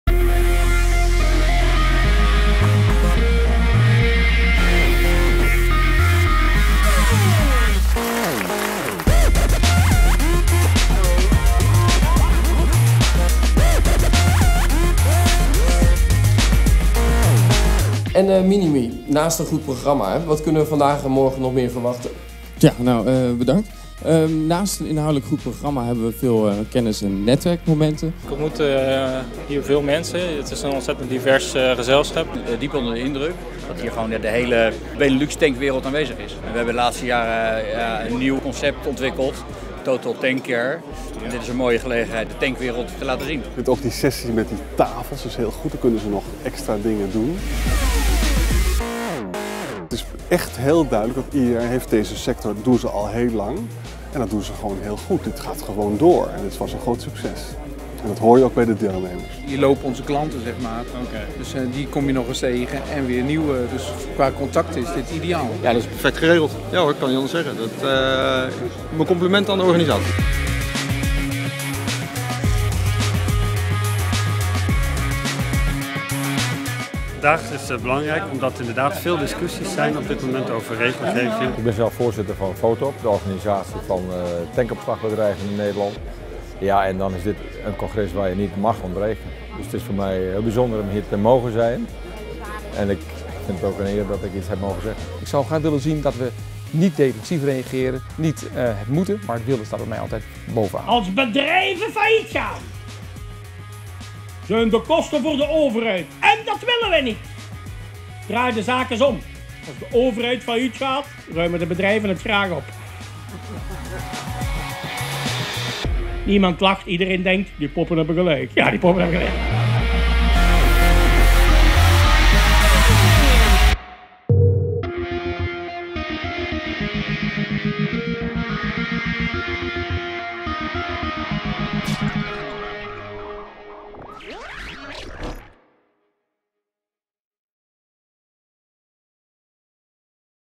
Opslagtanks 2014 Sfeerimpressie
Proef de sfeer tijdens de 24ste editie van de conferentie Opslagtanks.